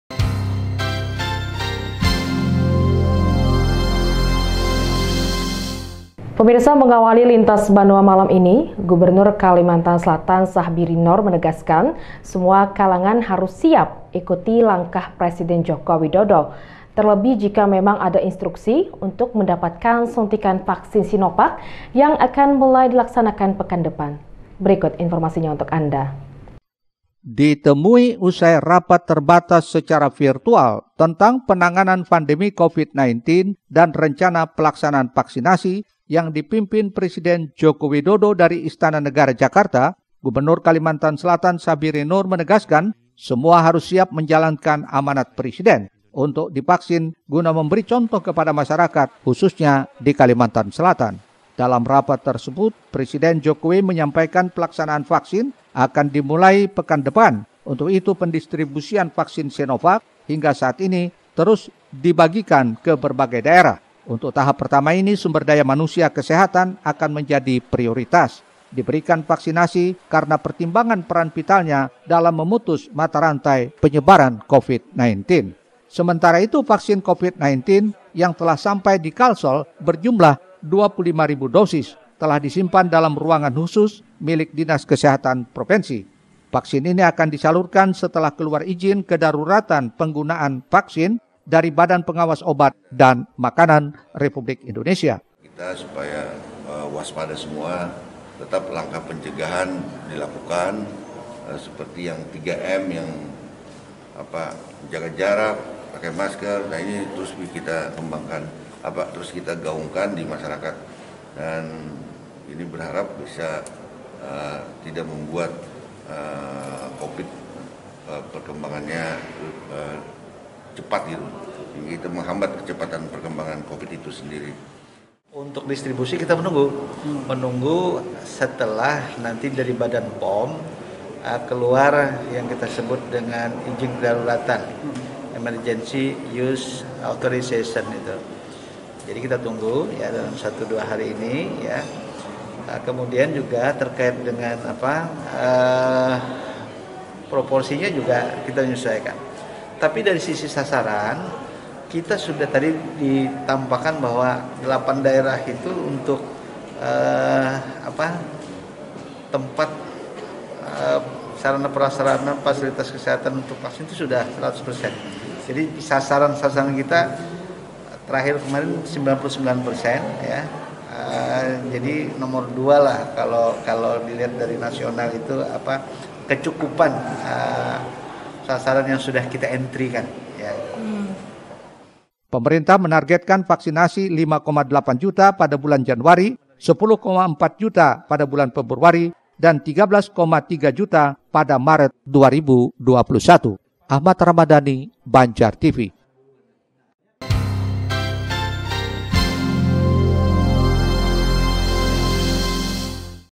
PAMAN BIRIN TEGASKAN SEMUA HARUS SIAP DIVAKSIN
Gubernur Kalsel, Sahbirin Noor, menegaskan semua kalangan harus siap ikuti langkah Presiden Joko Widodo. Terlebih jika memang ada instruksi untuk mendapatkan suntikan vaksin sinovac yang akan mulai dilaksanakan pekan depan.